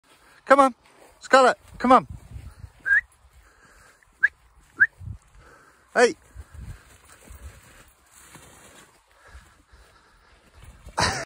Husky Frolics in the Field || ViralHog
Occurred on June 2022 / Lincolnshire

"Video of my husky dog called Scarlett. Taken during a country dog walk in Lincolnshire. Let her off the lead and she started pouncing in the field."